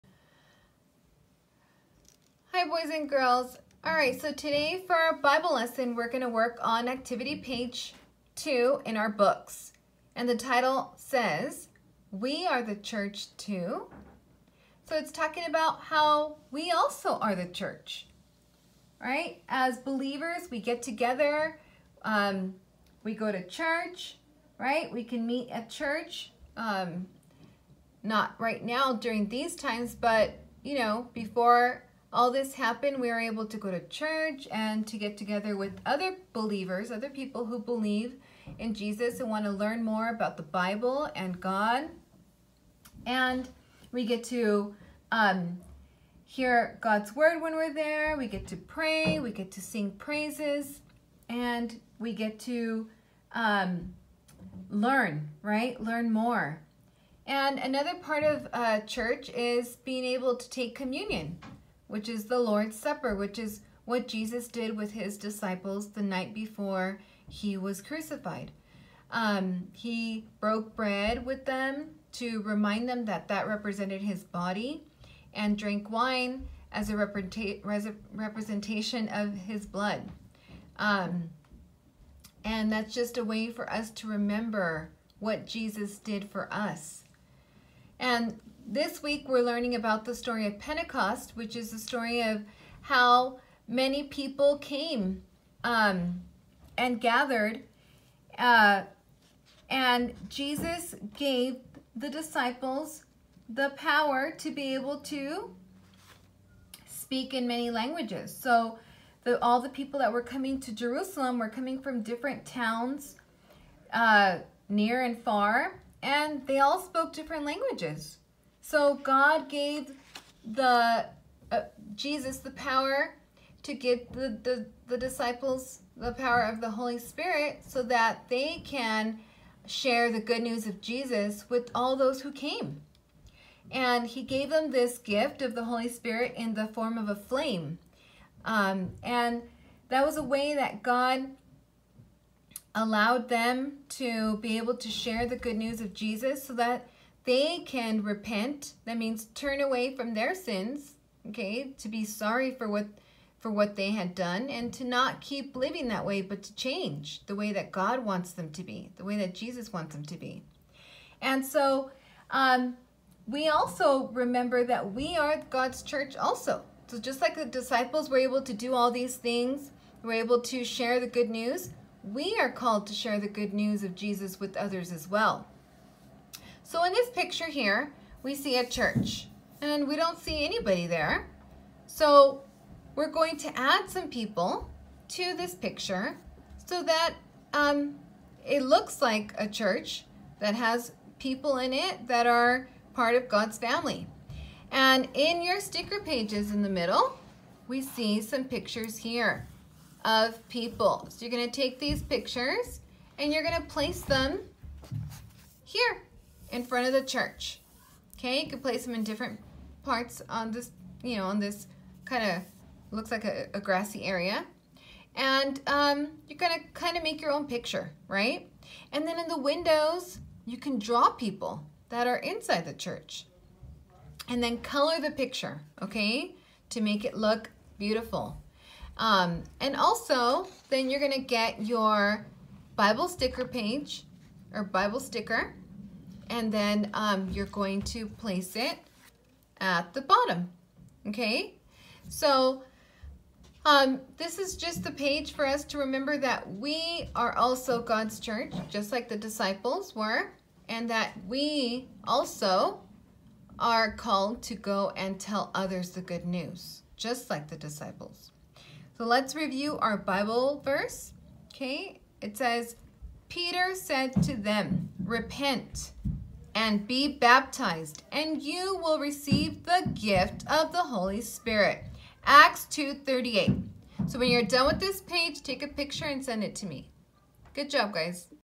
Tuesday 5/5/20 Bible Activity: “We Are the Church Too?”
In this video I talk about how we are the church just like the disciples were the church during Jesus’ time. I also review this week’s story about Pentecost and go over the Bible verse: Acts 2:38.